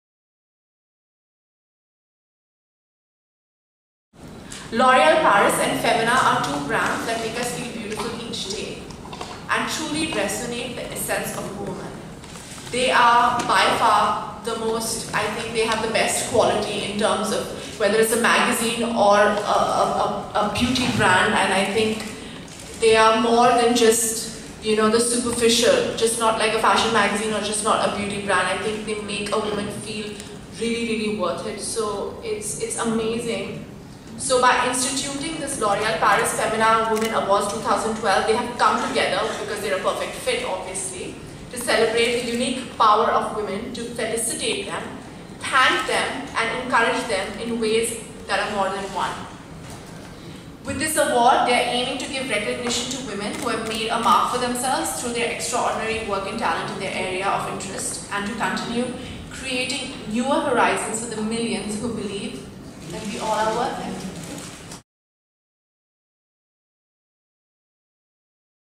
Sonam Kapoor talks about L'Oreal Paris-Femina Awards
Sonam Kapoor shares her views about L'Oreal and Femina as a brand.